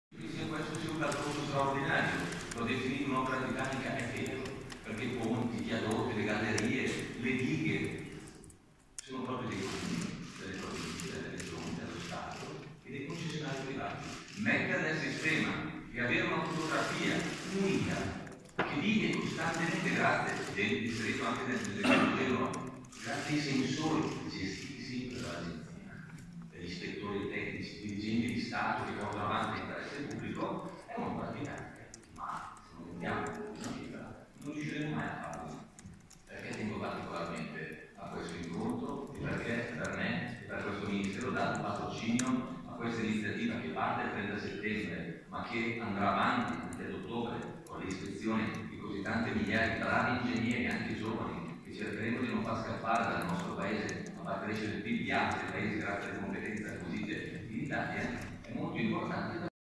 Toninelli parla del sisma bonus
Il ministro delle infrastrutture Danilo Toninelli parla del sisma bonus e della prevenzione sismica degli edifici in Italia. Ministero delle infrastrutture e dei trasporti 25 settembre 2018